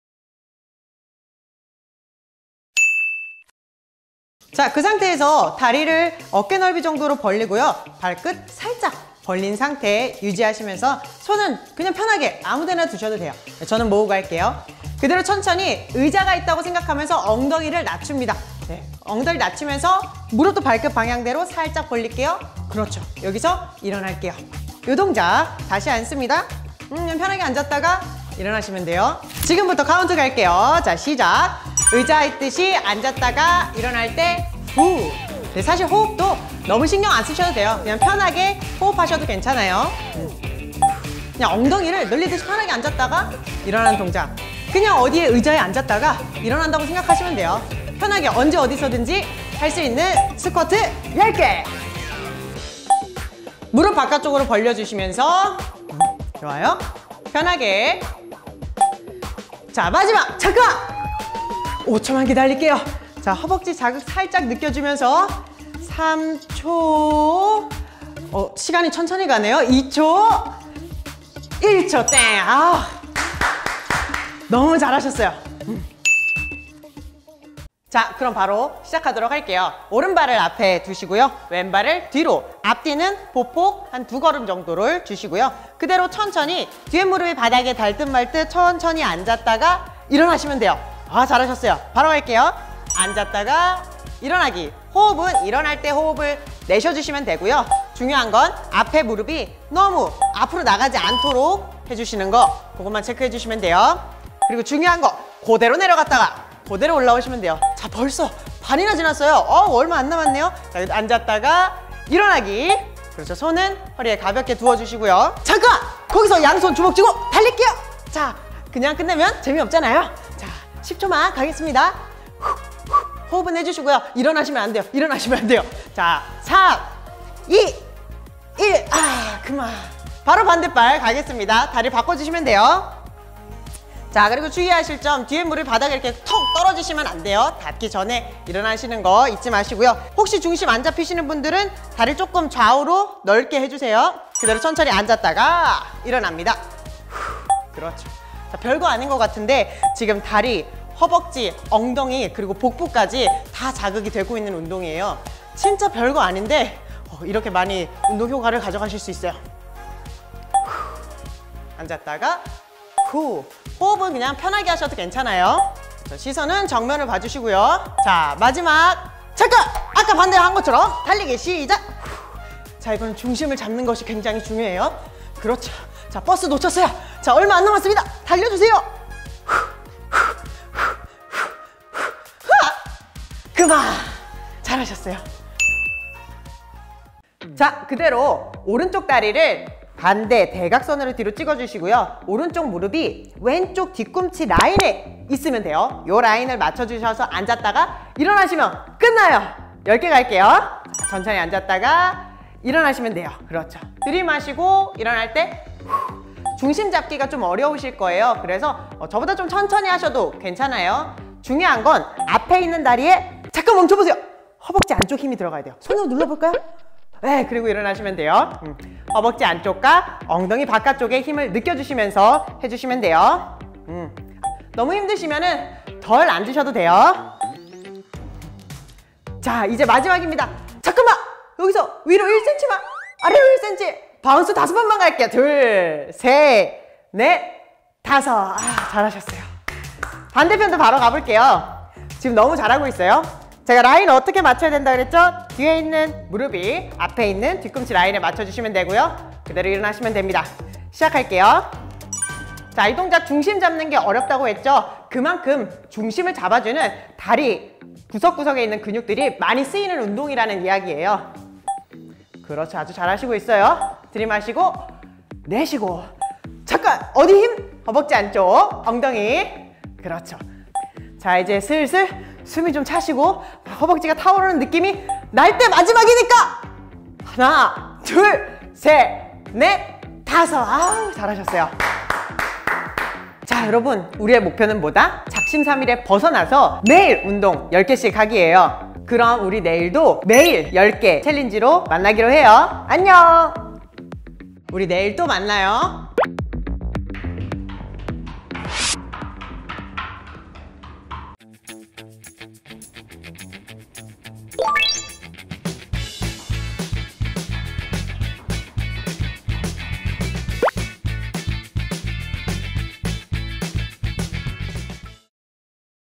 [매일 10개 챌린지 하체편] 다이어트 운동은 이걸로 끝! 5분으로 체지방 털업!!👊🏼ㅣ다노티비
안녕하세요, 다노블리 여러분! 😆
오늘은 엉덩이, 허벅지, 허벅지 안쪽 살을 깔끔하게 정리할 수 있는
매일 10개 챌린지 하체 통합편을 준비했어요!

요 영상들을 한 편에 담았어요!

매번 작심삼일로 끝나는 빡센(?) 다이어트 운동은 ❌
매일 10개 챌린지로 건강한 운동 습관 만들기 ⭕!

그럼 오늘부터 하루 5분으로
하체 지방을 활활 태워볼까요?! 🔥

▷goo.gl/nCCD5d